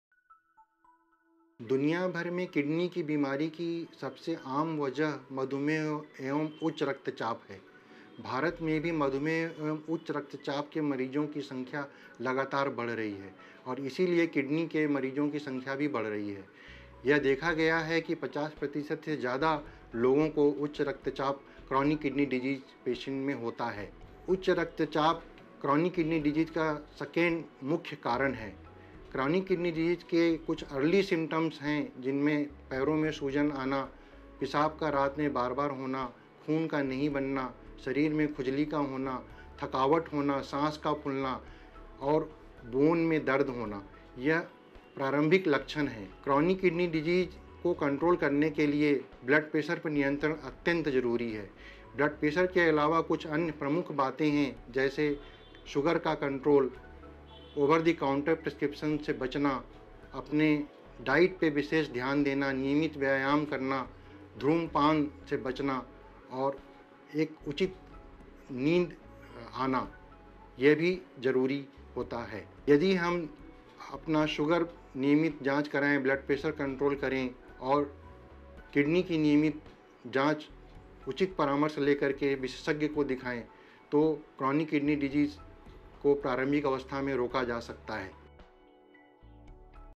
Dr. Anil Jain 2
A patient awareness message on Kidney Disease under the campaign “Tazloc Kidney Disease” by Dr. Anil Jain